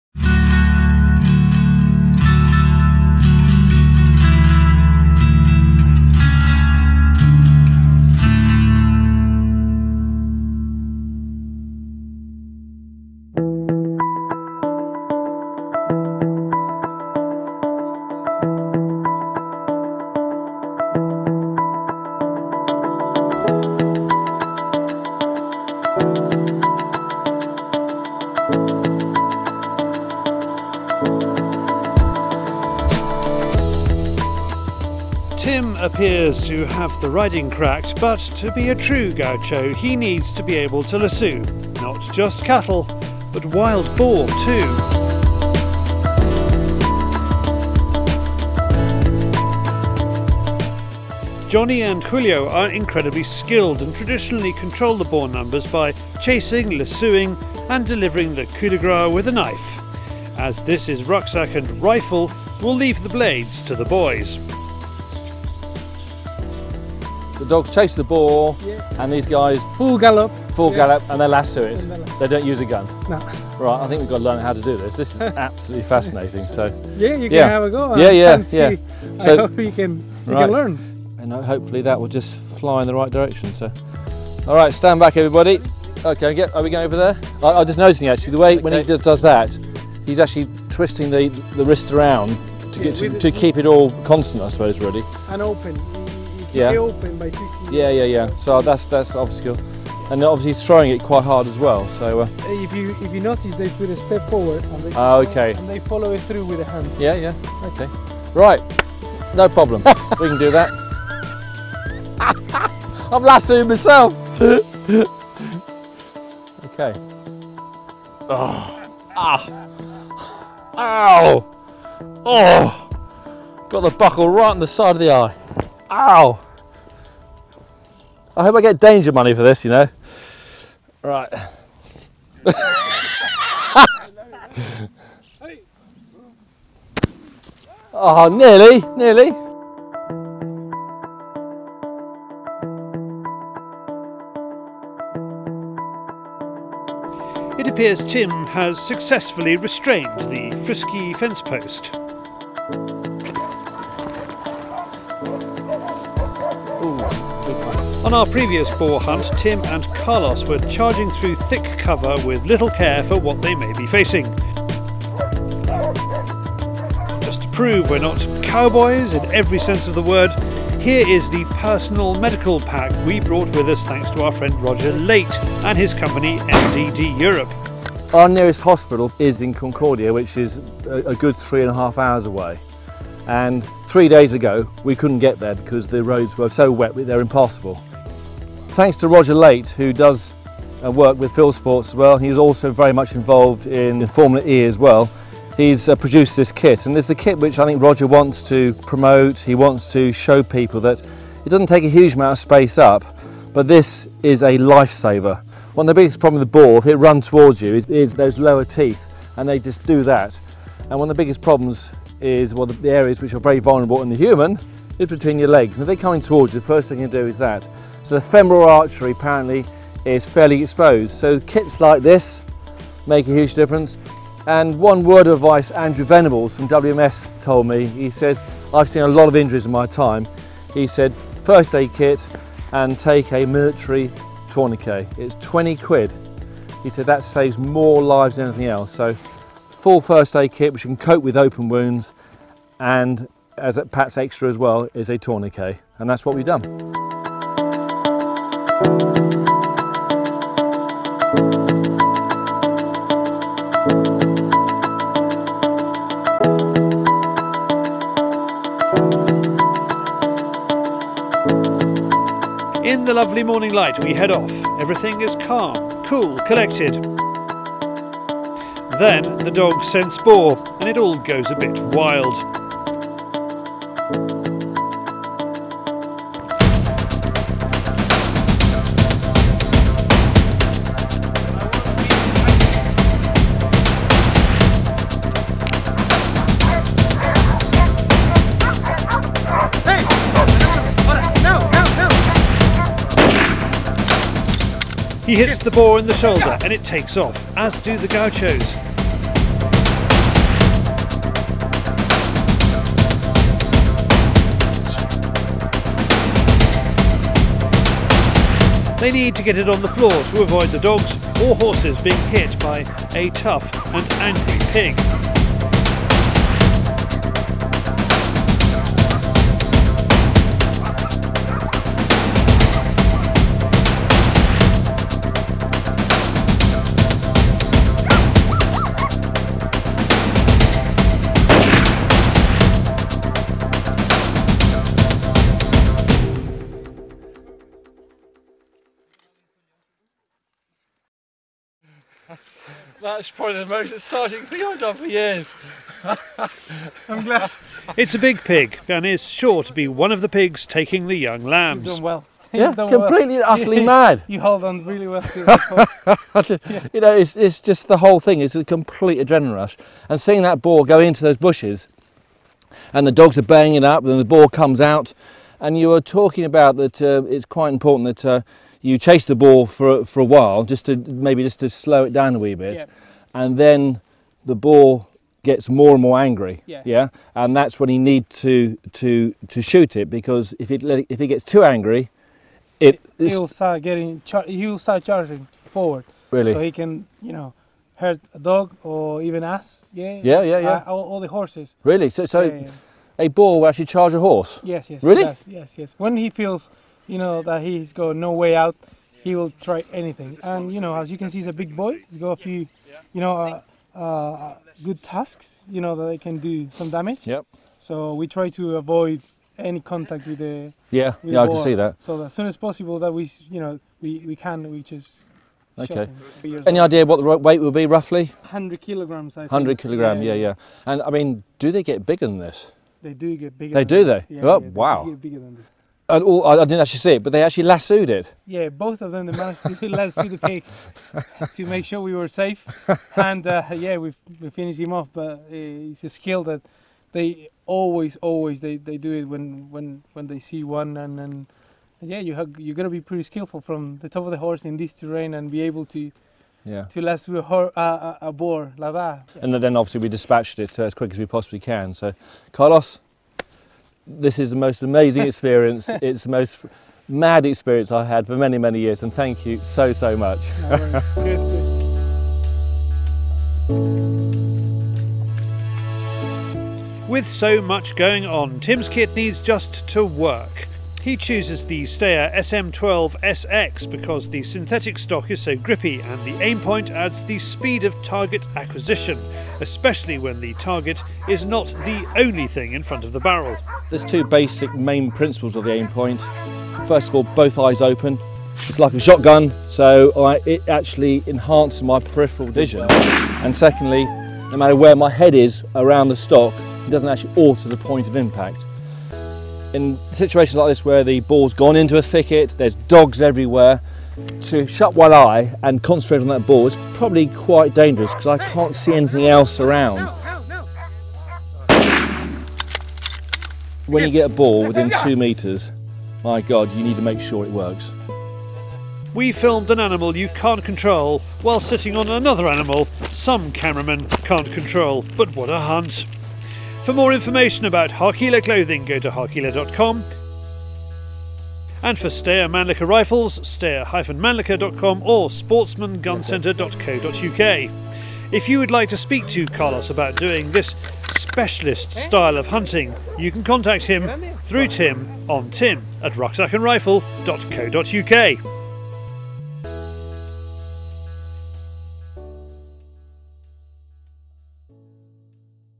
Running Boar Hunt
Here's hunting so-fast-so-furious: Tim Pilbeam is chasing down wild boar on horseback in Argentina, where they lassoo them and shoot them. He calls it the most exciting hunt he has been on. It is the latest in his Rucksack & Rifle series.

Why shoot wild boar?
Most of continental Europe has a wild boar problem. There are millions of animals causing possibly billions of euros of damage to farmland. Happily, there is a control system, based on sport hunting. In some countries, where sport hunters do not manage numbers effectively, they have to pay reparations to farmers.
Wild boar are omnivorous and approximately 400 species of plants and animals have been reported to be part of their diet. Their habit of rooting through the floor of woodland and pasture leaves a clear indicator of their presence. They take both eggs and nestlings of ground-nesting birds. Damage to agriculture can also be extensive and concerns have been raised regarding collisions with traffic. Boar have no natural predators apart from small populations of wolves, meaning culls are necessary to control population growth.
Wild boar are susceptible to the same diseases as domestic pigs and therefore have the potential to spread infectious disease such as swine fever, foot & mouth and Aujesky’s disease.